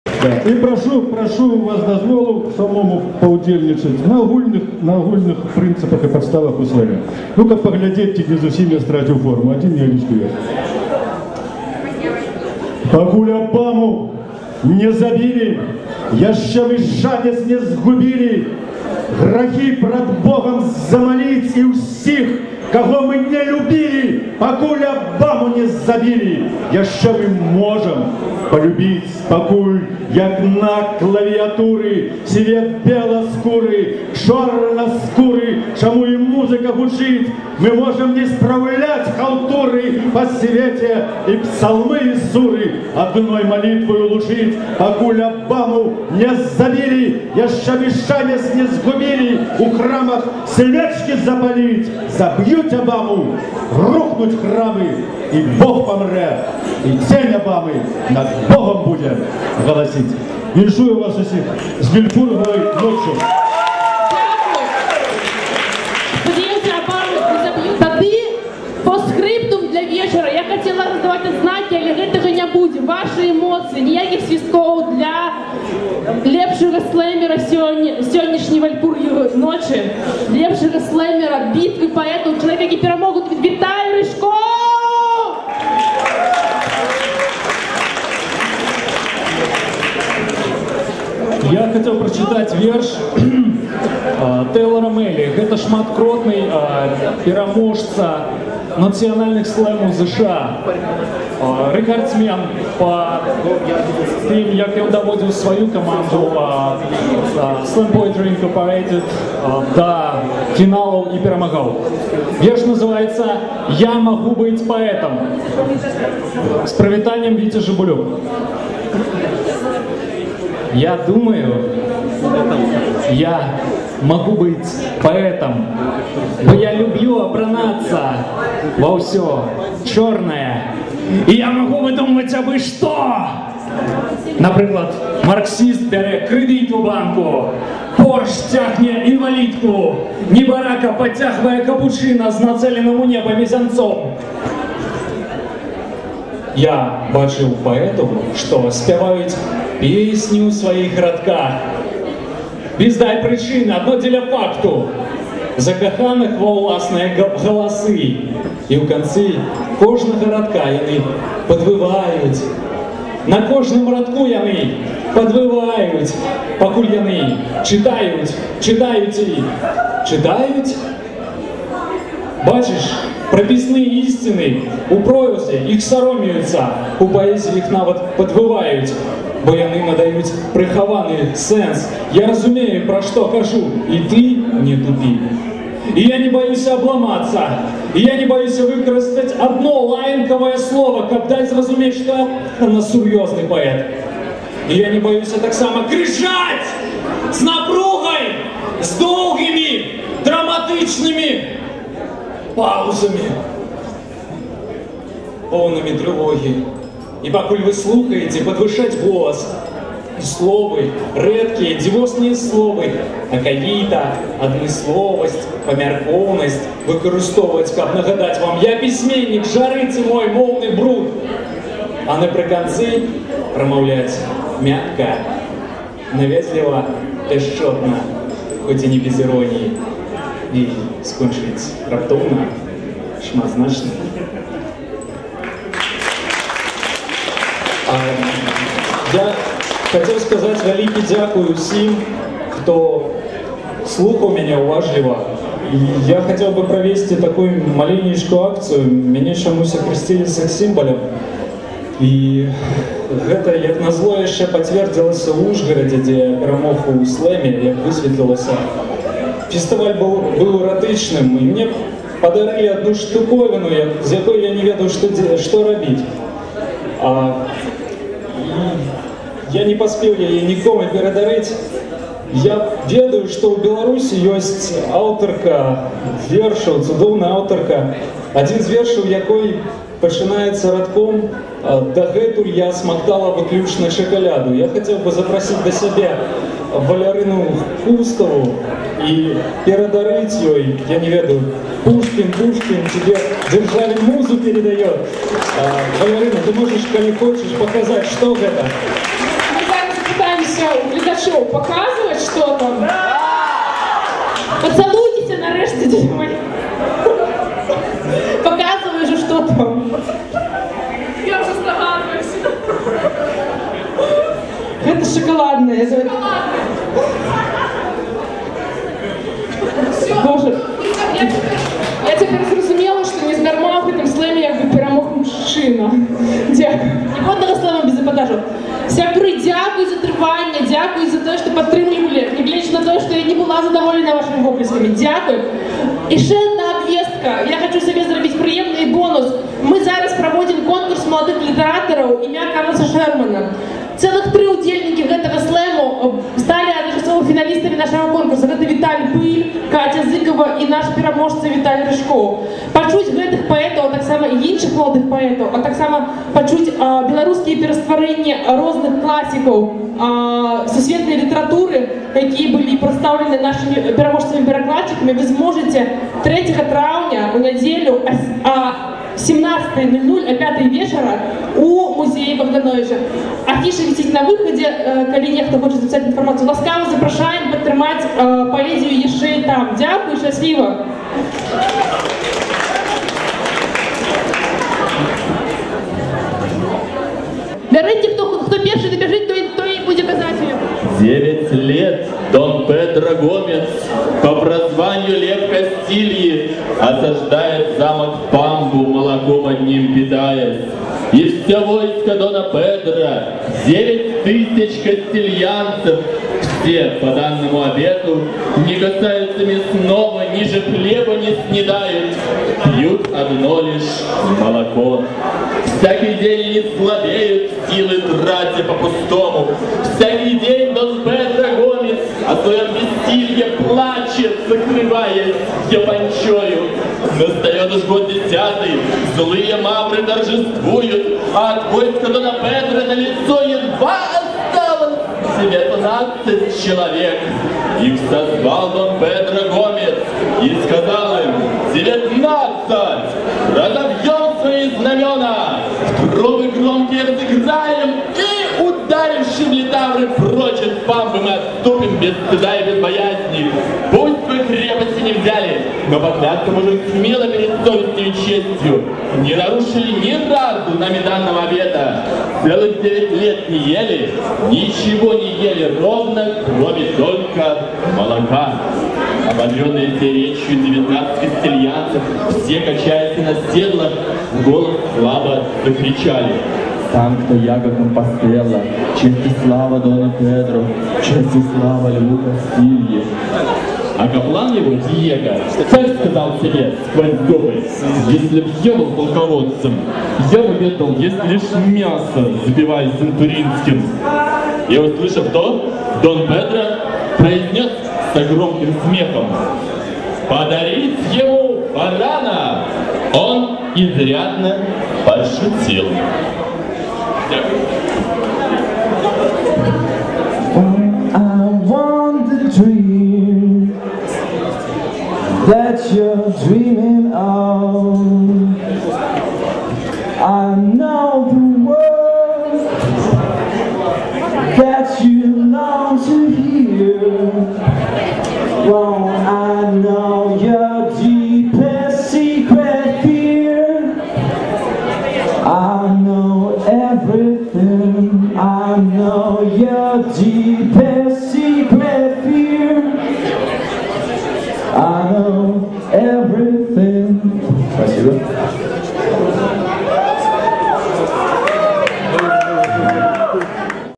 Битва Поэтов - третья часть
30 апреля в минском ресторане Яблоко в рамках проекта Fun House прошел поэтический слем "Битва поэтов".